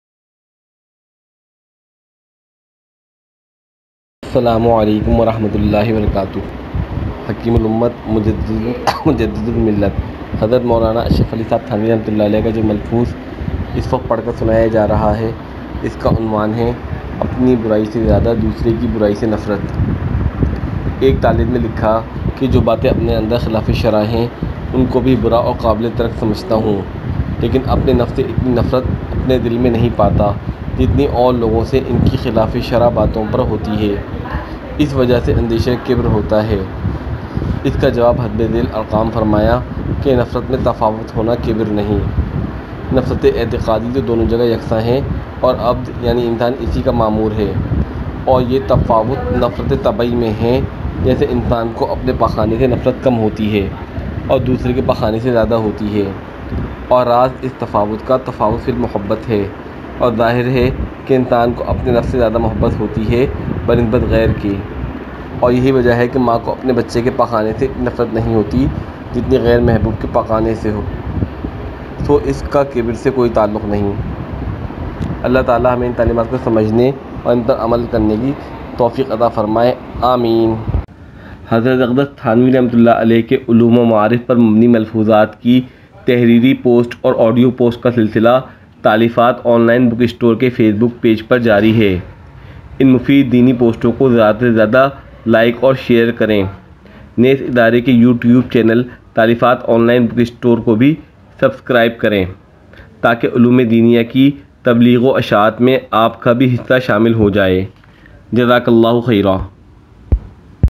Apni Burai se ziyada doosry ki burai se nafrat (Malfoozat E Hakeem Ul Ummat RA)
Malfoozat and Sayings of Hazrat Maulana Ashraf Ali Thanvi RA.
Voice by : Muhammad Faisal Sahib (Khalifa of Maulana Abdul Mateen Sahib Bangladeshi)

Taleefat Online Bookstore.